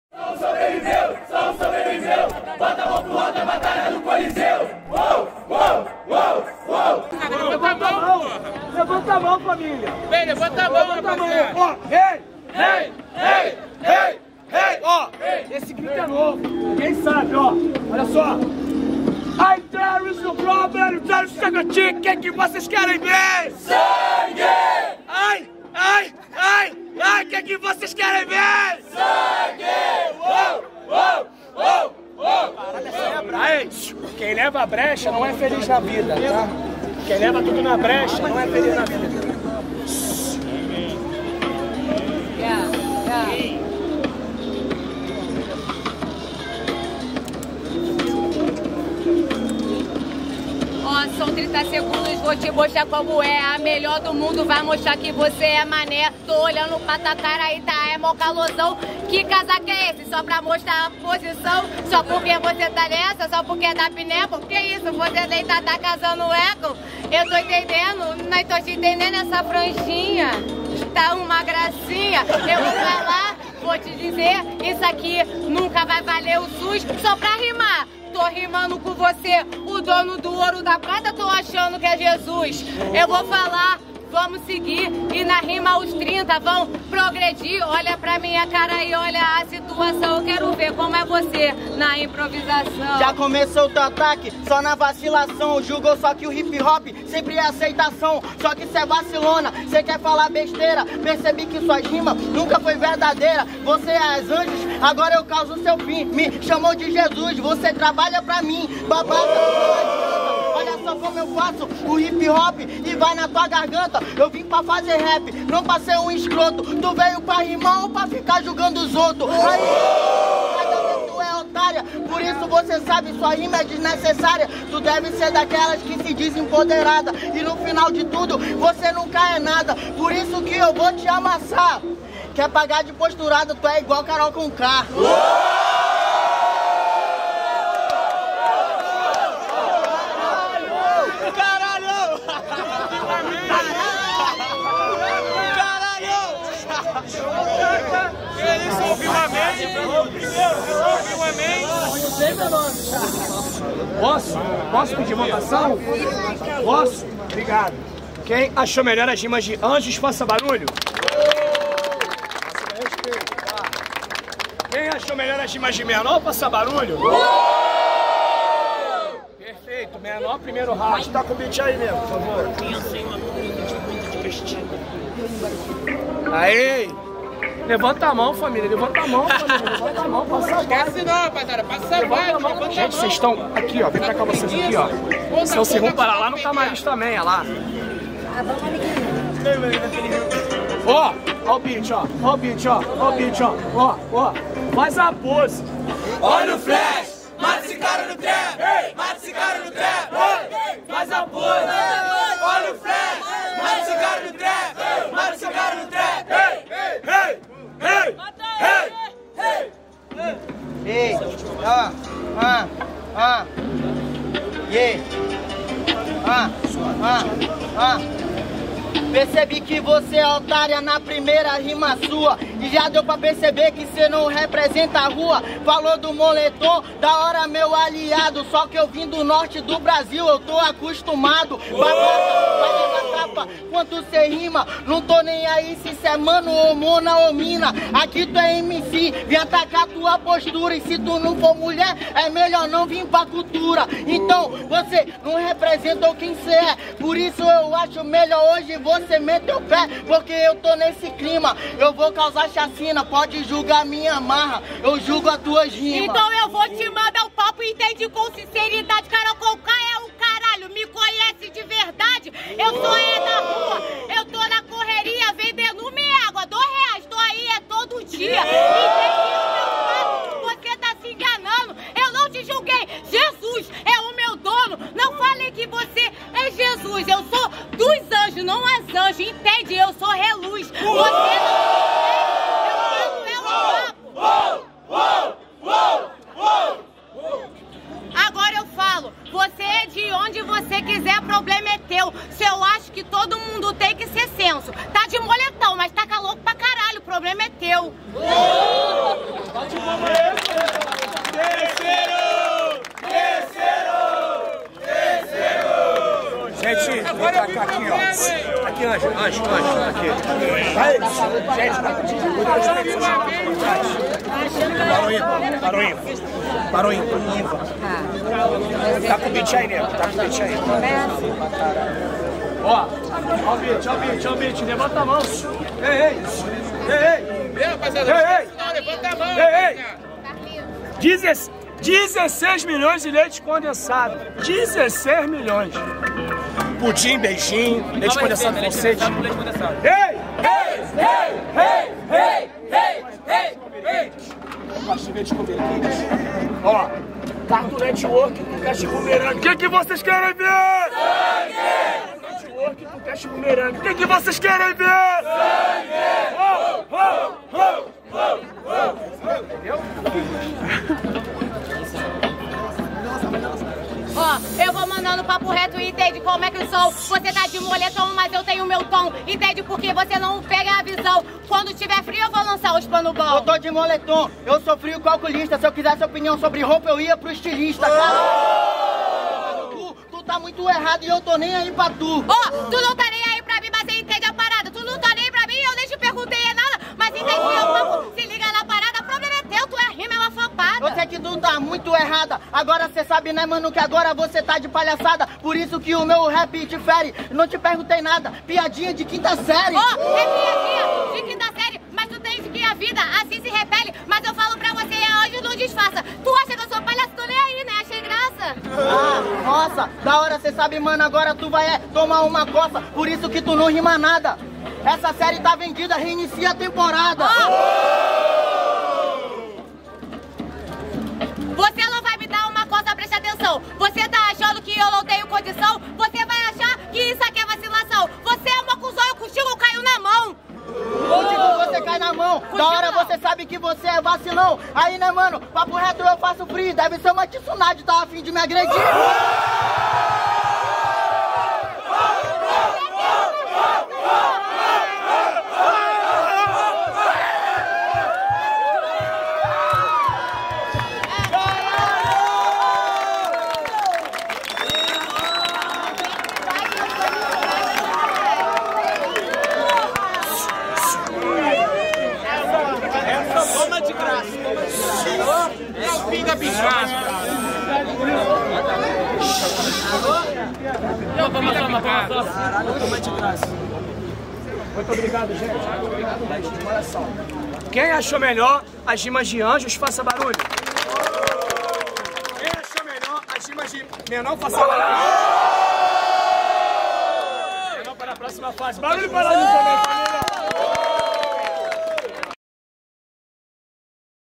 MENOR (AM) X ANJOS - PRIMEIRA FASE - BATALHA DO COLISEU - EDIÇÃO 68
Siga no Instagram: @Bdcoliseu

Batalha do Coliseu acontece toda Quarta Feira, na Praça Agripino Griecco no Méier.